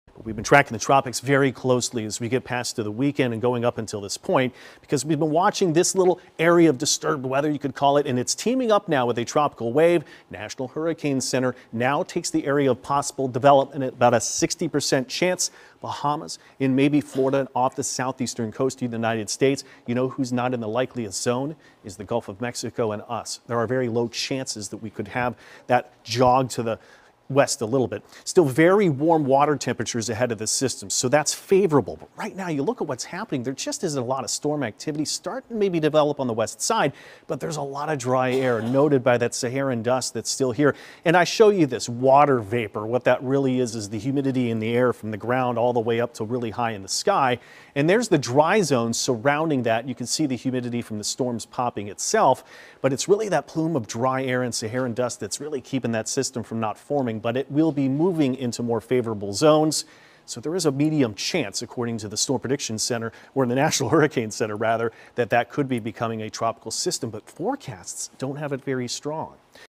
Tracking the tropics and who's likeliest to be impacted
Meteorologist Devon Lucie tracks a system forecast to have a medium chance, 60%, of development over the next 3-7 days, examining where the system is now and what the conditions are out ahead of it that are more favorable to support development and finally where the likeliest track of that system will take.